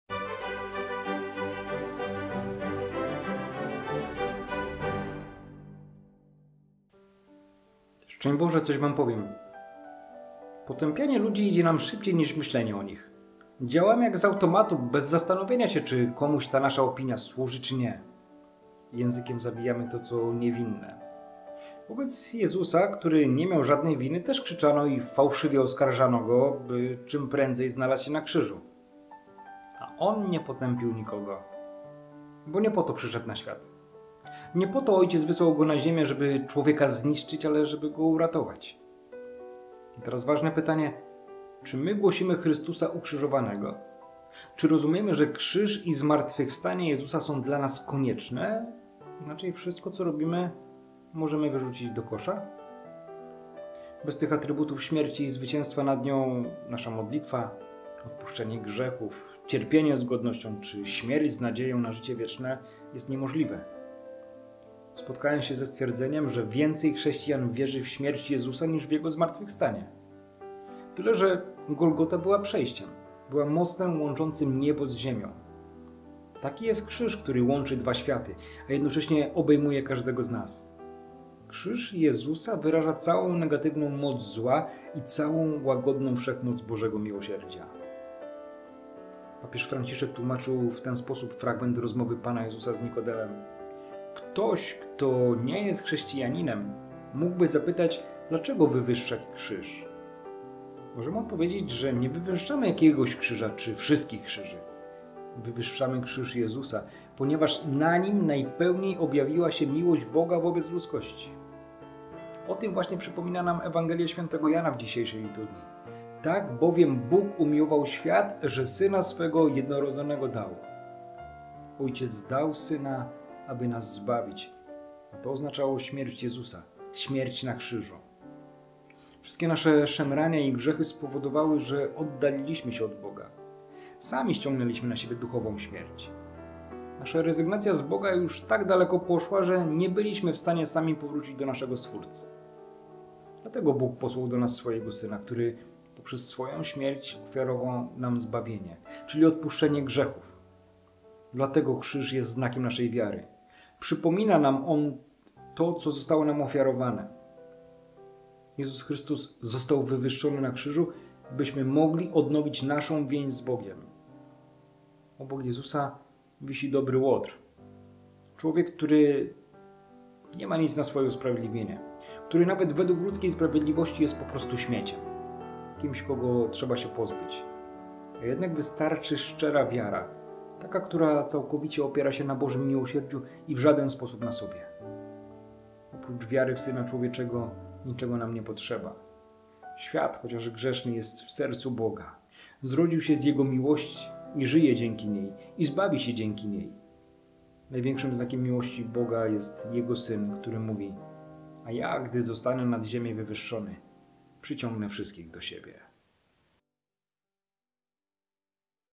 coswampowiem J 3, 13-17 || 14 września 2022
Potępianie ludzi idzie nam szybciej niż myślenie o nich. Działamy jak z automatu, bez zastanowienia się, czy komuś nasza opinia służy czy nie. Językiem zabijamy to, co niewinne.